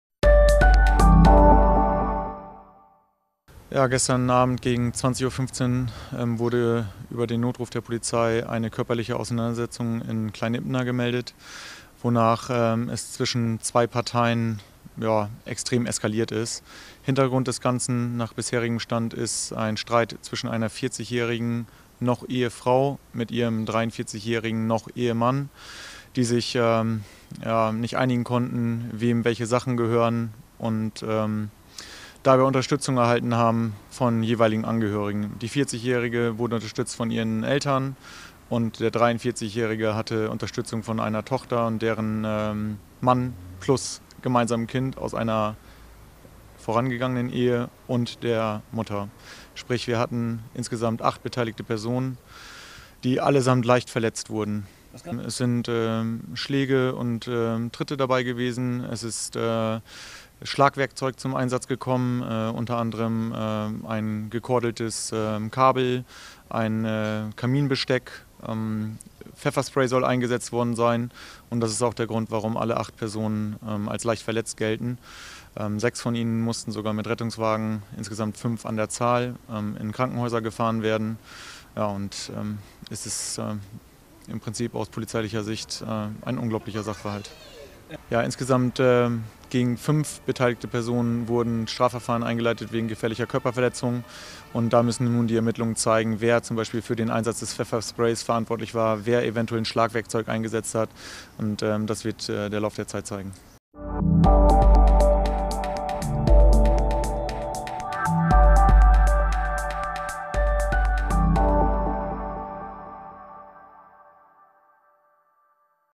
Beziehungsstreit bei Groß Ippener eskaliert
Familienstreitigkeiten sind am Mittwoch, 5. Juni, in Groß Ippener eskaliert. Um 20.15 Uhr gingen zwei Notrufe bei der Polizei ein, in denen von jeweils beteiligten Personen geschildert wurde, dass es zu einer körperlichen Auseinandersetzung zwischen zwei Personen gekommen war. Insgesamt waren acht Personen in diesen Streit verwickelt. Unter anderem kamen Pfefferspray und Schlagwerkzeuge zum Einsatz.